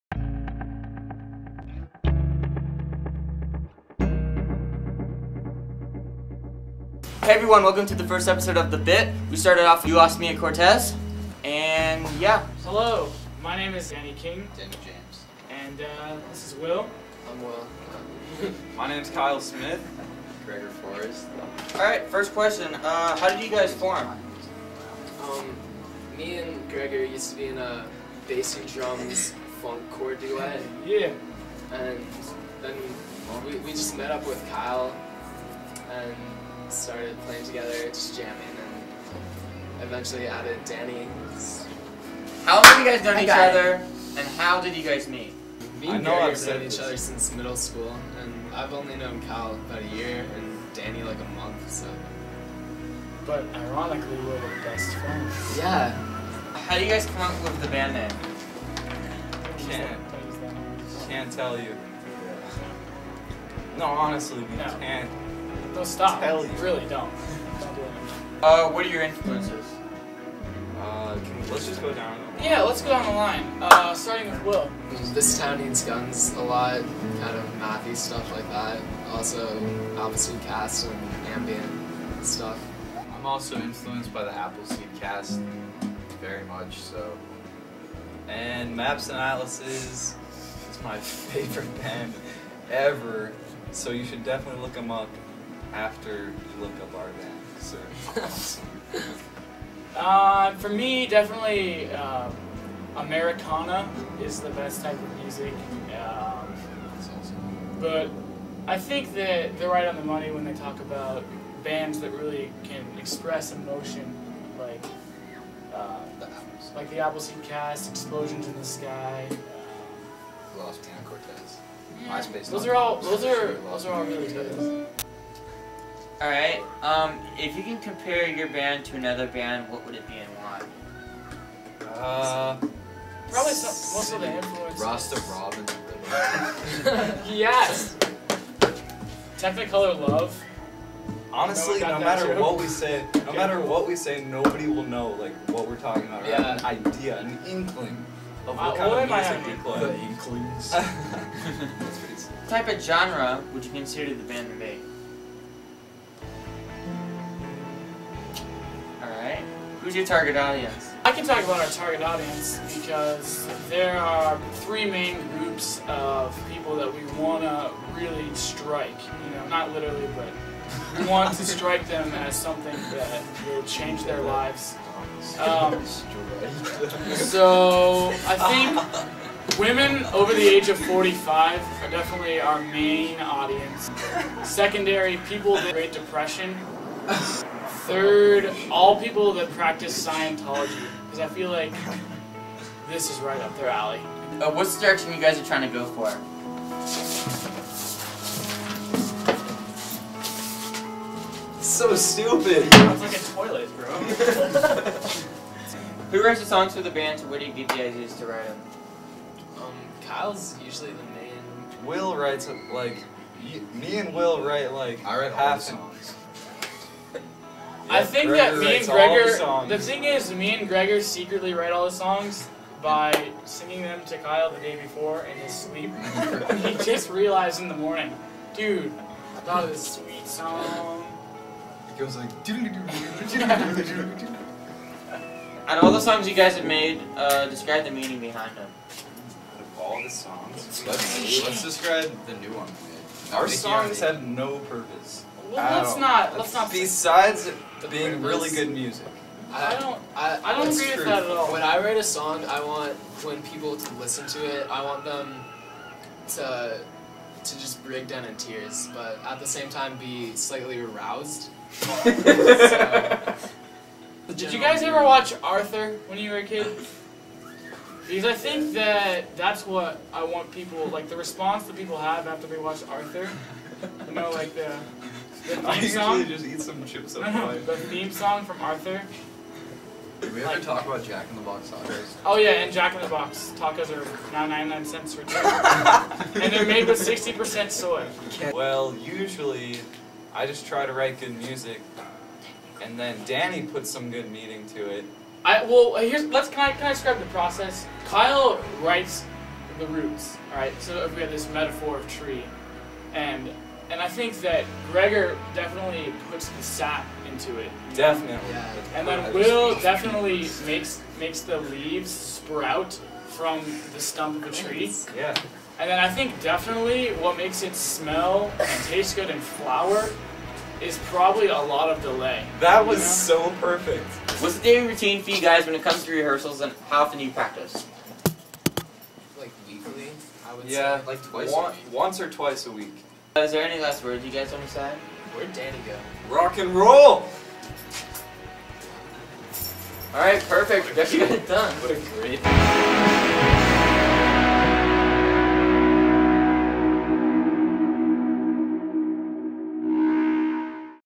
You Lost Me At Cortez Interview
This has to be my favorite out of the ones i've done. It's more chill and the 2nd half is really funny with some cool effects. this was originally shot a while ago like around October back around the time when they first formed.